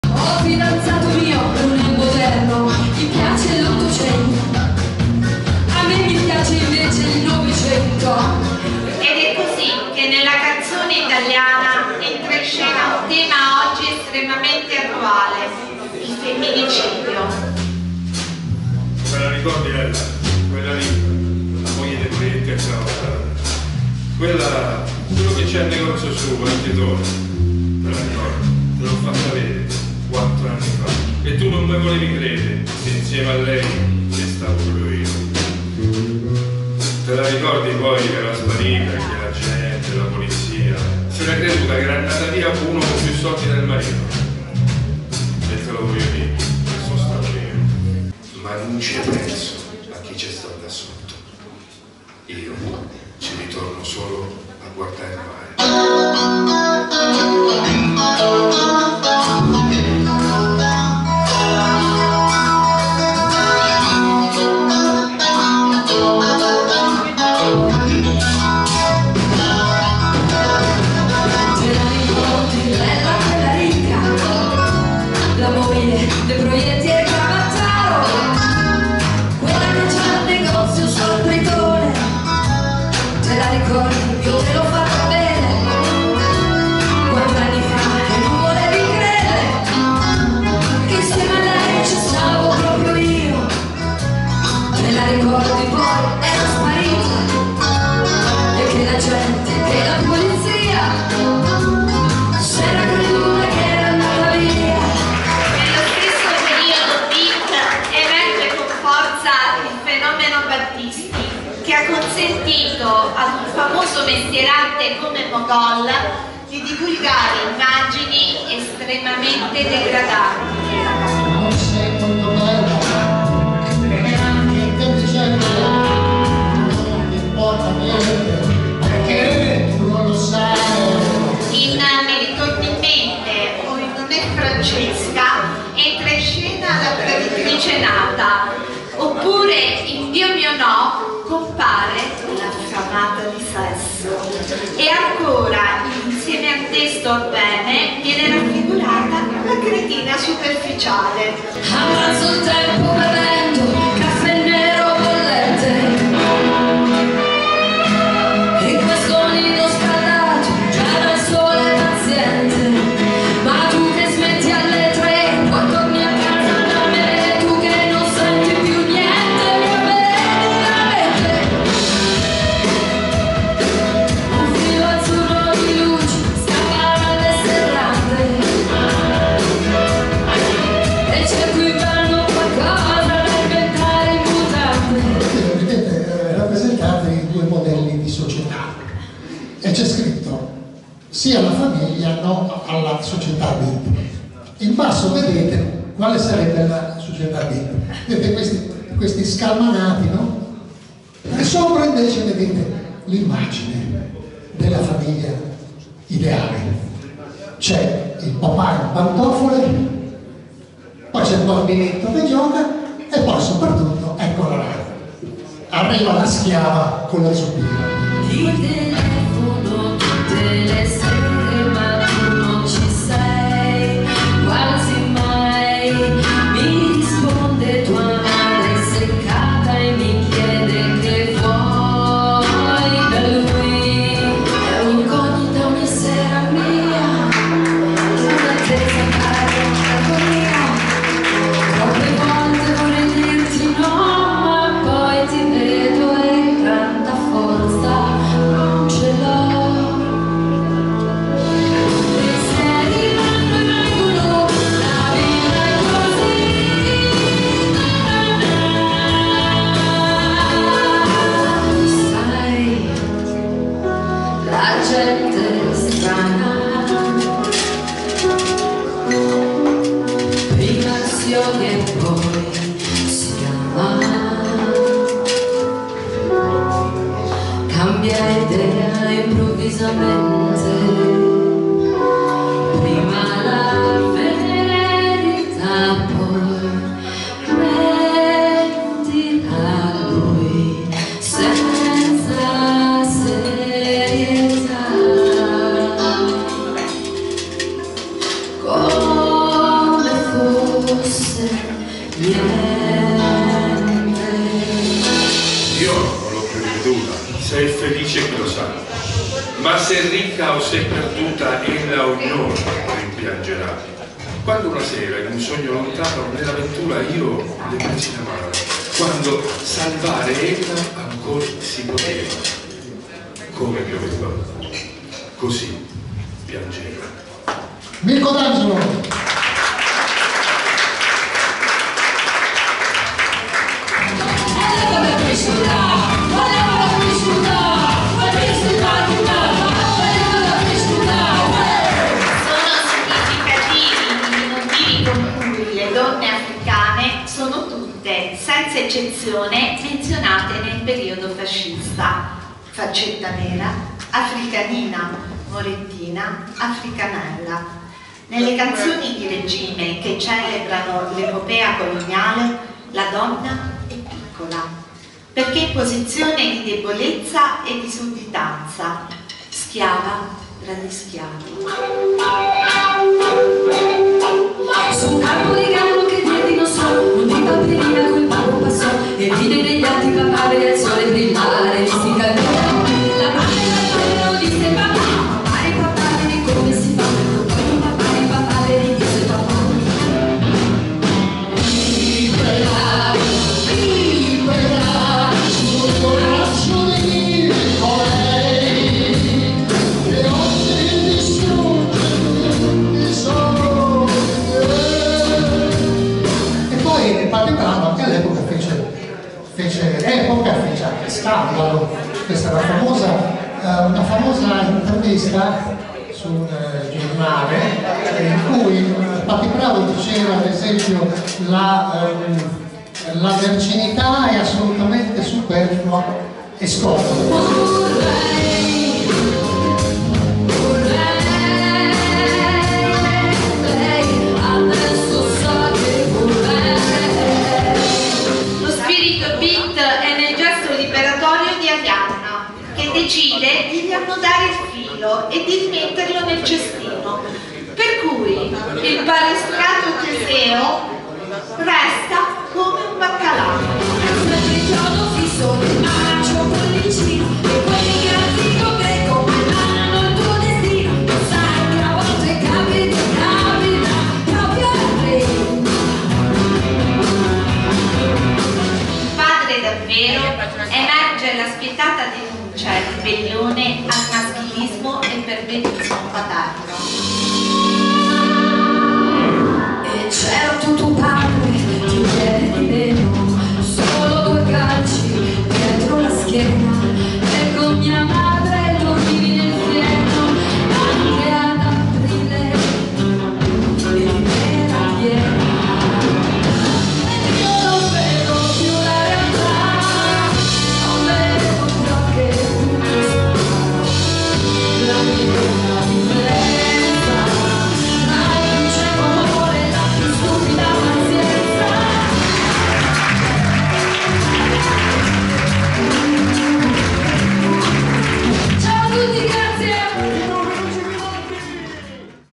La Donna nella Canzone Italiana del Novecento - Trailer 2023
Prende forma la nuova edizione 2023 dello spettacolo scritto da Umberto Bultrighini "La Donna nella Canzone Italiana del Novecento".
Performance eseguita dallo storico gruppo beat "Tubi Lungimiranti", con la partecipazione di Milko D'Angelo e Rita Fazio.